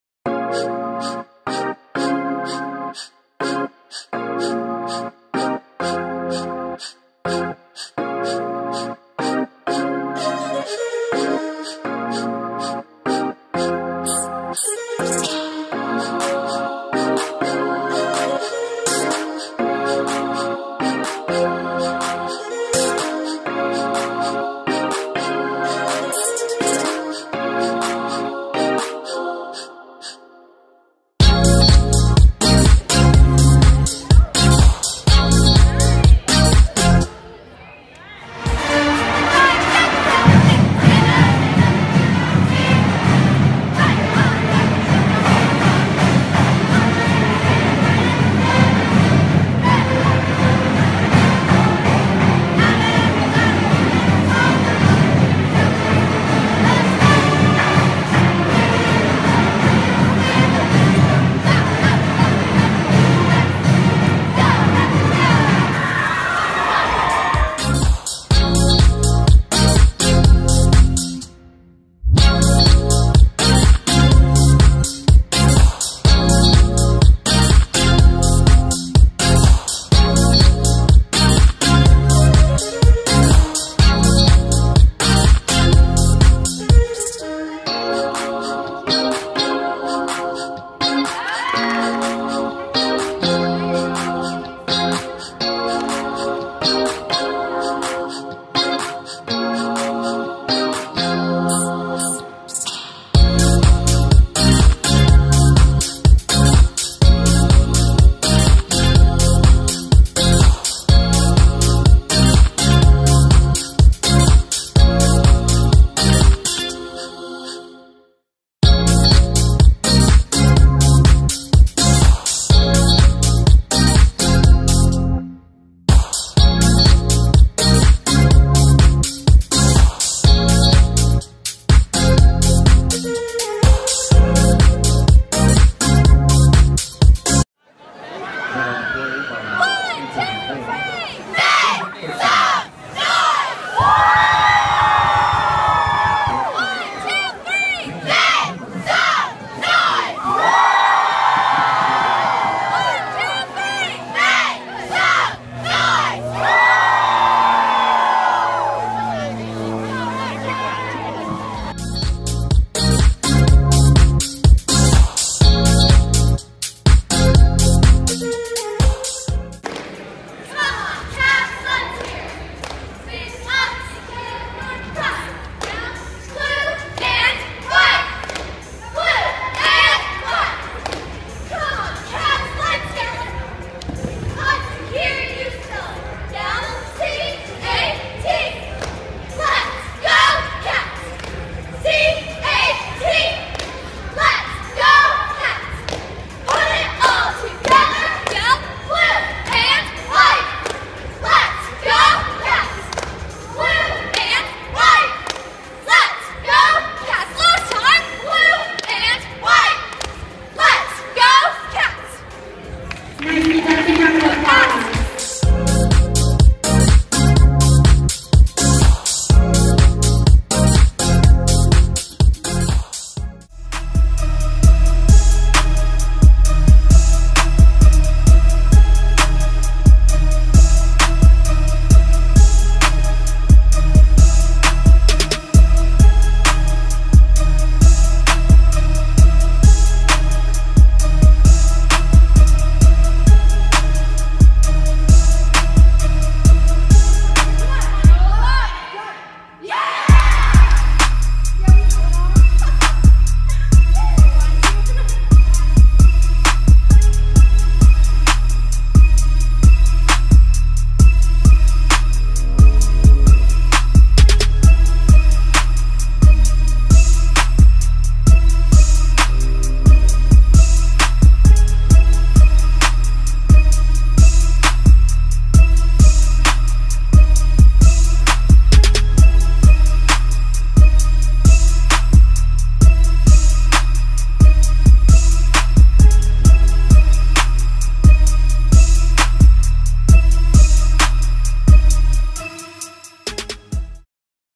Fiona McIntosh
Fiona McIntosh cheerleading highlight film. 2015 5A cheerleading state champion. 2014, 2015, 2016 UCA national finalist. Team Leader. Class of 2016.